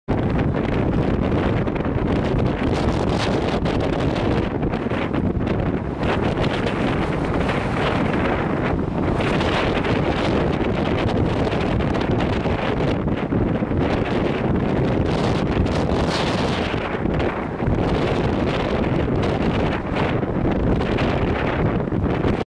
Evening High Tide - 03/01/14
It looked like a God Damn Town Meeting
Police closed off a section of the sea wall walkway